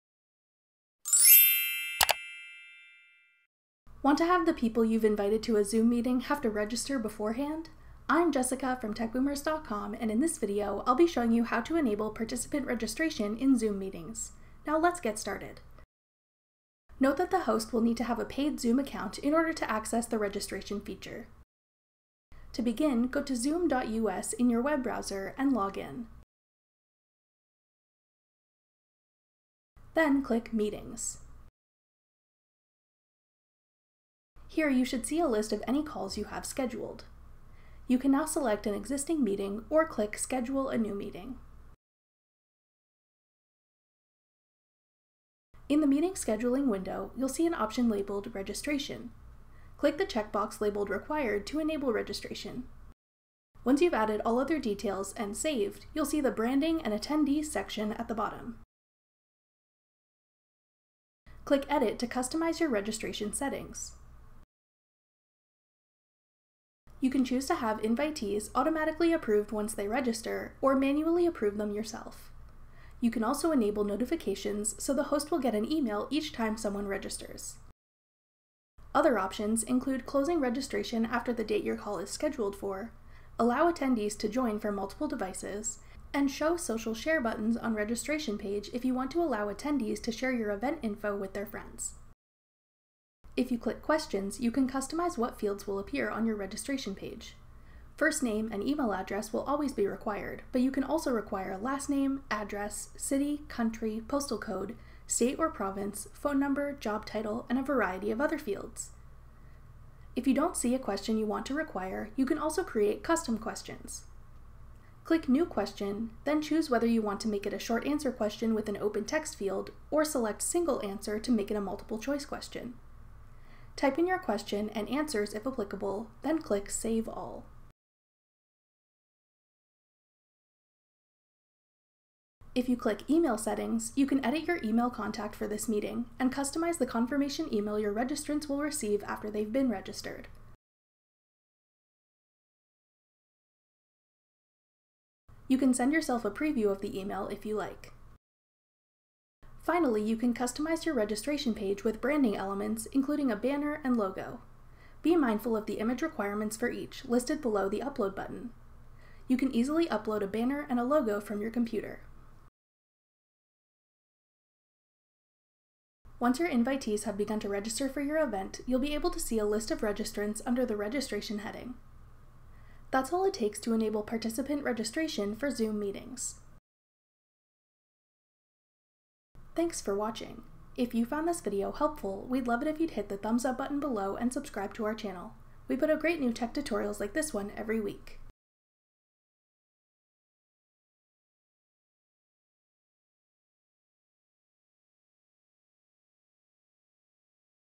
How to Enable Participant Registration in Zoom Meetings
Want to have the people you’ve invited to a Zoom meeting have to register beforehand? Watch this tutorial video to learn how to enable registration for Zoom meetings.

NOTE: The host of the meeting will need to have a paid Zoom account in order to access the registration feature.

To begin, go to Zoom.us in your web browser and log in. Then click Meetings. Here you should see a list of any calls you have scheduled. You can now select an existing meeting, or click Schedule a New Meeting. In the meeting scheduling window, you’ll see an option labelled Registration. Click the check box labelled “Required” to enable registration. Once you’ve added all other details and saved, you’ll see the Branding and Attendees section at the bottom. Click Edit to customize your registration settings. You can choose to have invitees automatically approved once they register, or manually approve them yourself. You can also enable notifications so the host will get an email each time someone registers. Other options include closing registration after the date your call is scheduled for, allow attendees to join from multiple devices and show social share buttons on registration page if you want to allow attendees to share your event info with their friends. If you click Questions, you can customize what fields will appear on your registration page. First name and email address will always be required, but you can also require last name, address, city, country, postal code, state/province, phone number, job title, and a variety of other fields. If you don’t see a question you want to require, you can also create custom questions. Click New Question, then choose whether you want to make it a short answer question with an open text field, or select Single Answer to make it a multiple choice question. Type in your question and answers if applicable, then click Save All.

You can send yourself a preview of the email if you like.

Finally, you can customize your registration page with branding elements including a banner and logo. Be mindful of the image requirements for each, listed below the upload button. You can easily upload a banner and a logo from your computer.

Once your invitees have begun to register for your event, you’ll be able to see a list of registrants under the Registration heading.